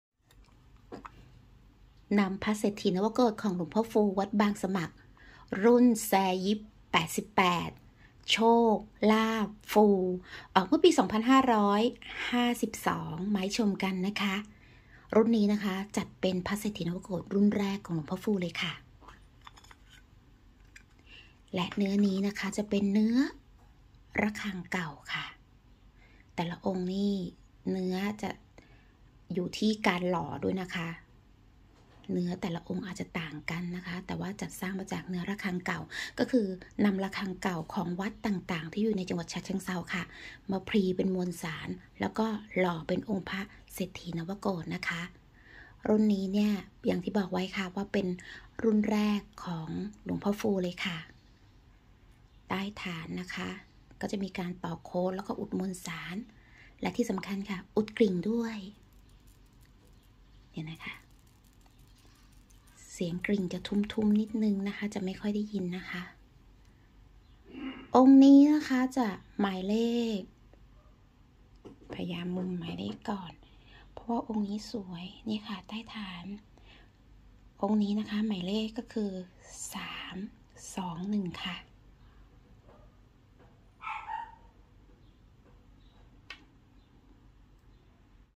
พระเศรษฐีนวโกฏิ รุ่นแรก รุ่นแซยิด 88 ปี โชค ลาภ ฟู หลวงพ่อฟู วัดบางสมัคร จ.ฉะเชิงเทรา ปี52
พระเศรษฐีนวโกฏิ รุ่นแรก รุ่นแซยิด 88 ปี โชค ลาภ ฟู หลวงพ่อฟู วัดบางสมัคร จ.ฉะเชิงเทรา ปี52 เนื้อระฆังเก่า

ที่ระลึกงานบุญคล้ายวันเกิดหลวงพ่อฟู ในวันที่ 3 ธันวาคม 2552

สนใจ @Line มาได้นะคะ
จะได้ถ่ายคลิบ/ถ่ายภาพให้เลือก
หรือโทรมาสอบถามได้ที่